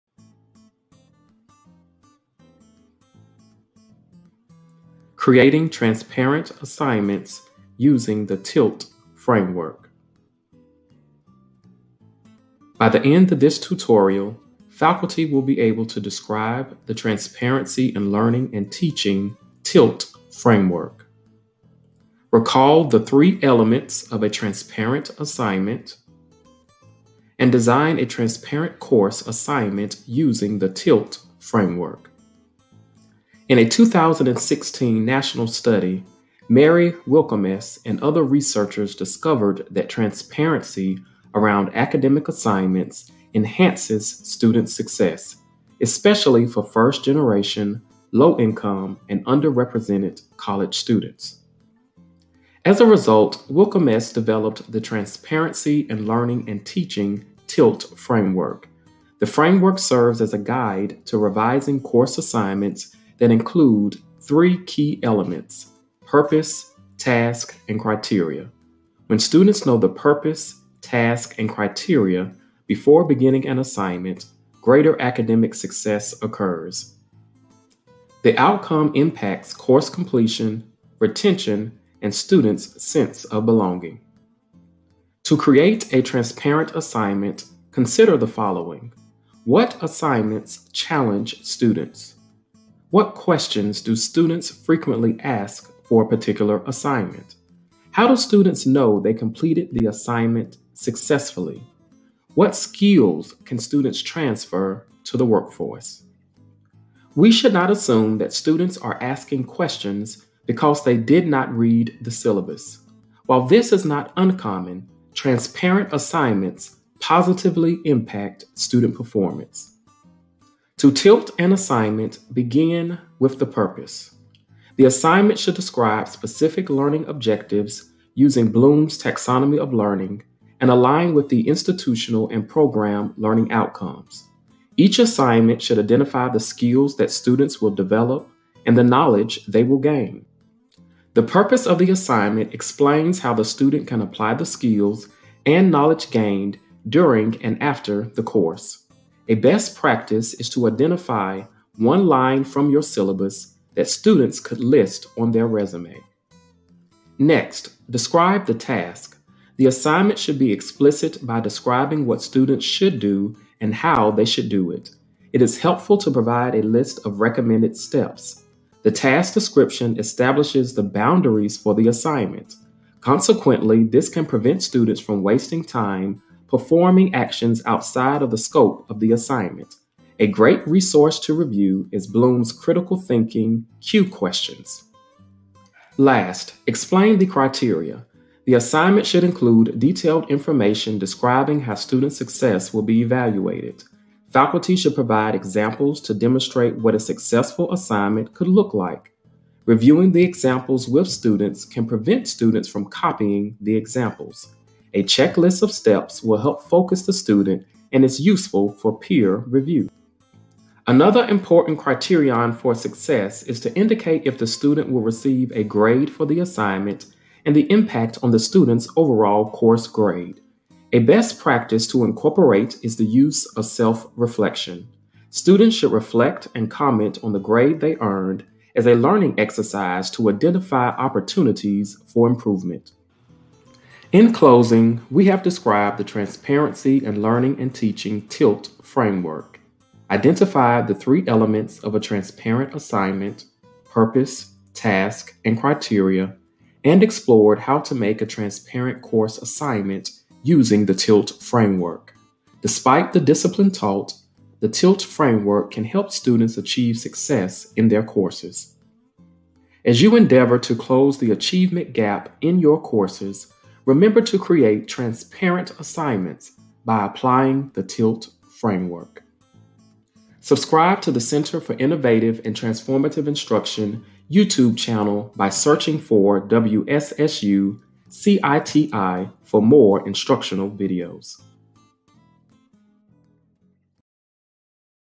Creating Transparent Assignments Using the TILT Framework
By the end of this tutorial, faculty will be able to describe the Transparency in Learning and Teaching (TILT) Framework, recall the three elements of a transparent assignment, and design a transparent course assignment using the TILT Framework.